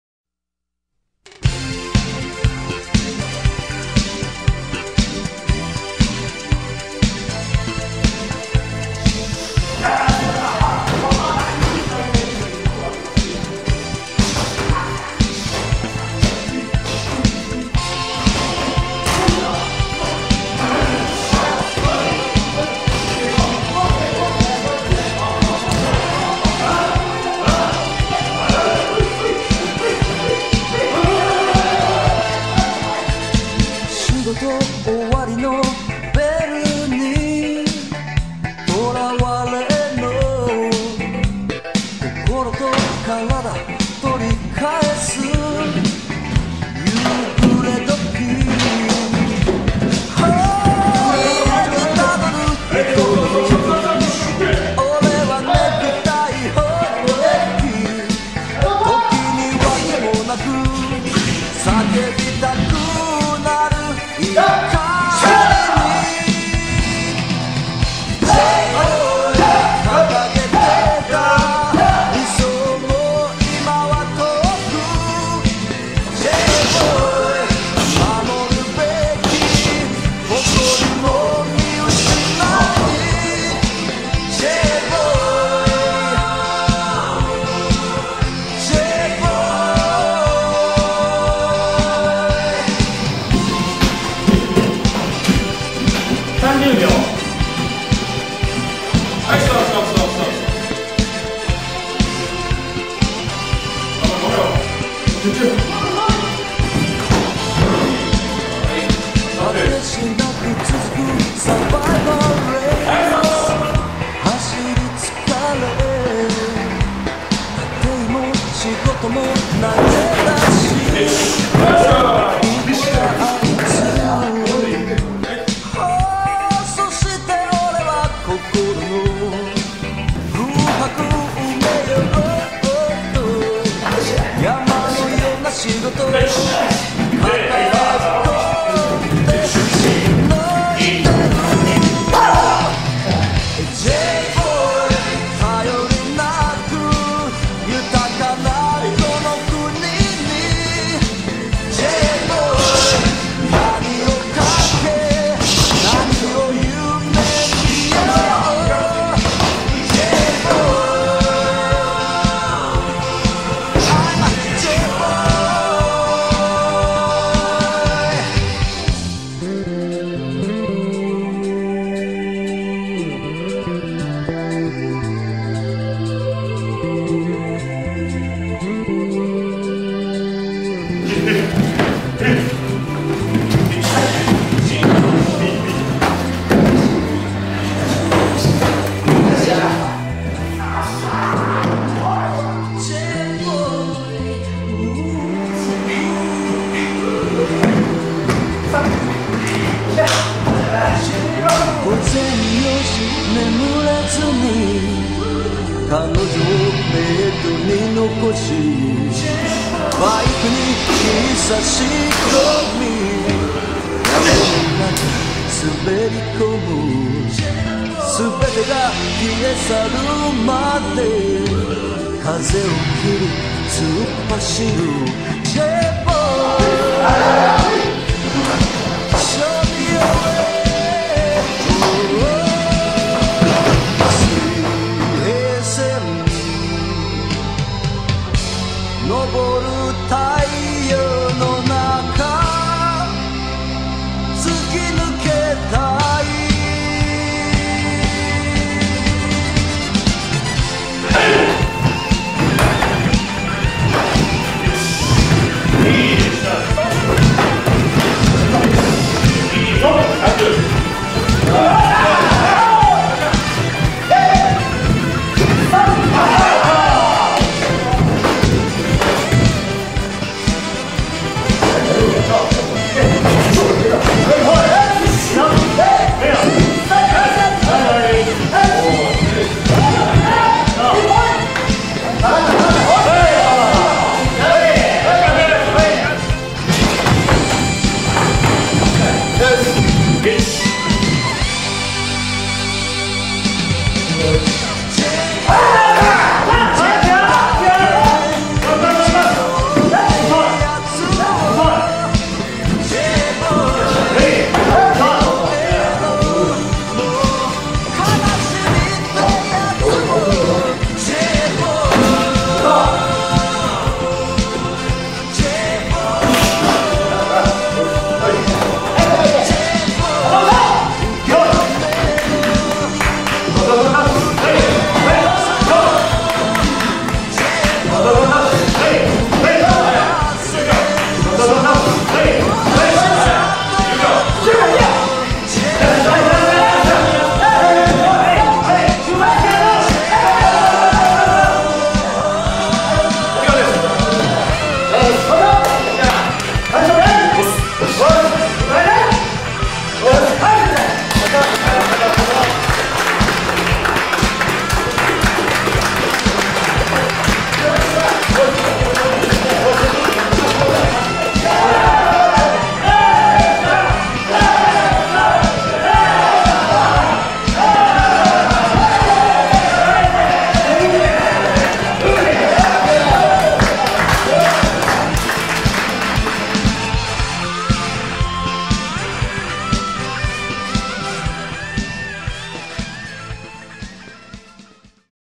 円心空手 のび太の黒帯物語 (Enshin Karate)
円心空手に入門して４年、全くの初心者であった三笠隼人が黒帯を取るまでの記録。

彼の体格を含めた成長ぶりと十人組手での気迫、そして十人目を相手にした時に見せた気合は必見。